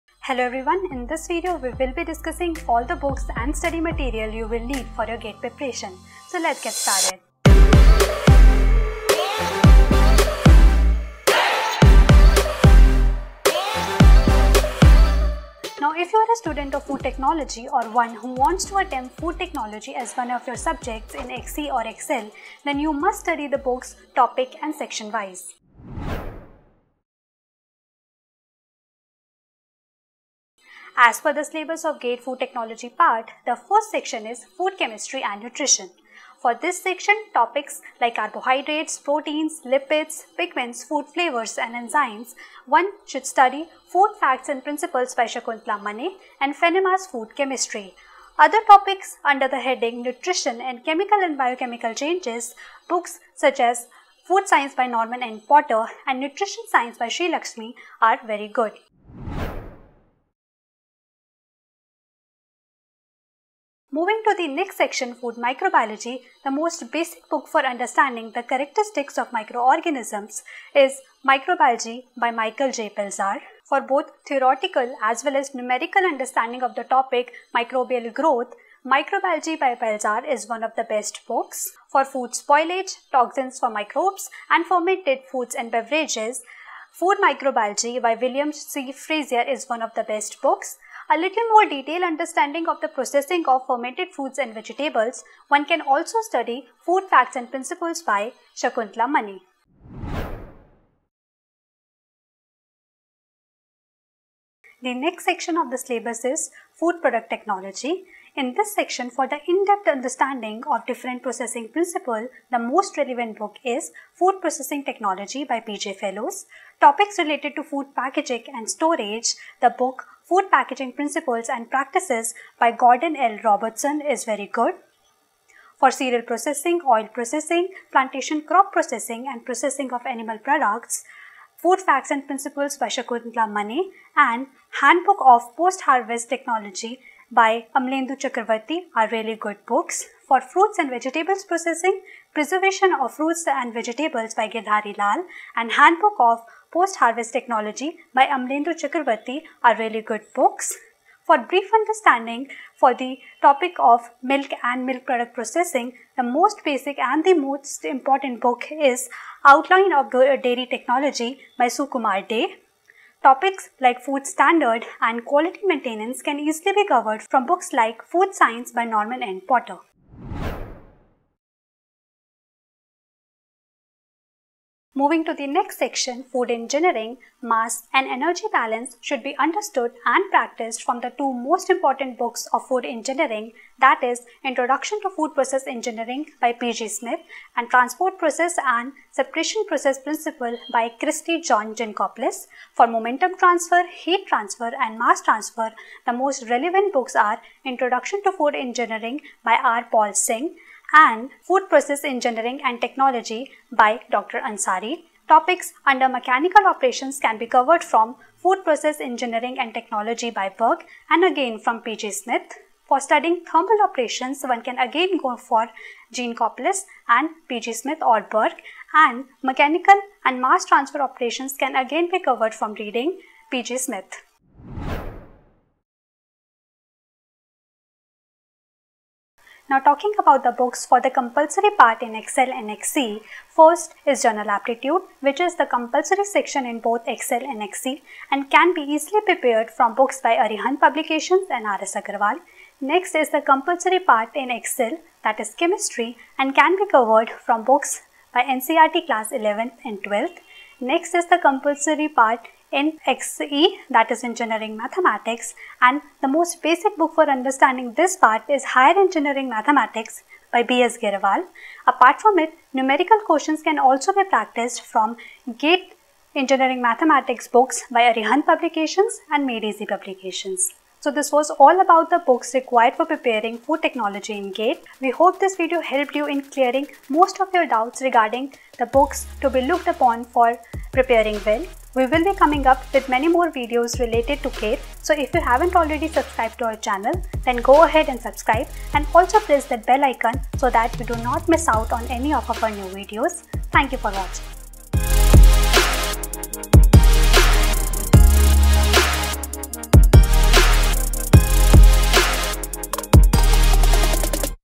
Books for GATE Preparation | Food Technology | XE | XL
This video is Part 4 of a series of videos where we will discuss all you need to know about the GATE exam preparation. In this video, we will discuss all the books and study material you would need for GATE preparation for Food Technology (XE and XL) according to the syllabus, section and topic-wise. We will also discuss the books for compulsory subjects in both XE and XL, i.e. Engineering Mathematics and Chemistry.

*CHAPTERS*
0:00 Introduction
0:25 Food Chemistry and Nutrition
0:58 Food Microbiology
1:40 Food Products Technology
2:50 Food Engineering
3:44 Books for Compulsory Sections